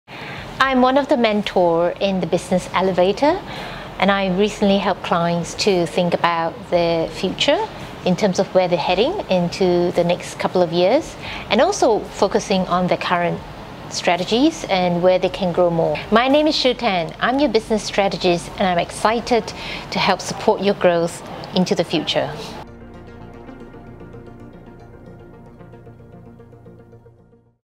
Business Elevator Shu
The Business Elevator is a group of experts in different areas, including myself, who come together as a mentor team. So you get just-in-time expertise on your business strategy and planning, digital marketing and sales approach, mindset shifts, people and culture, and money management and finance. We also have a trusted team of suppliers that we refer you to so you don't have to waste time searching for them yourself.